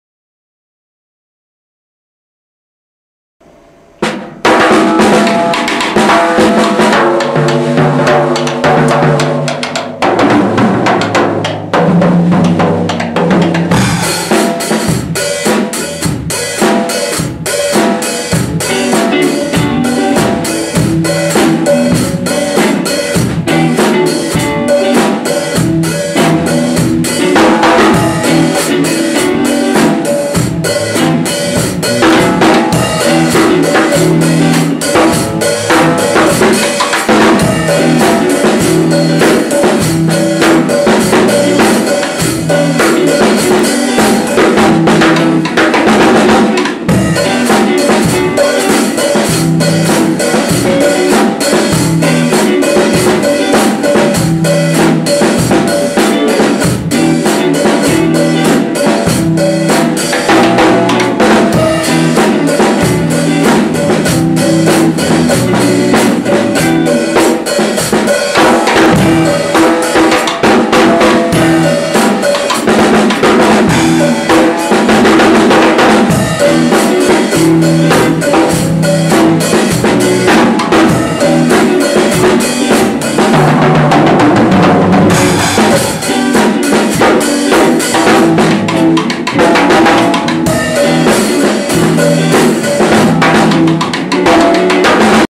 Impro batterie par Alex
Alex après 1 an de batterie en juin 2006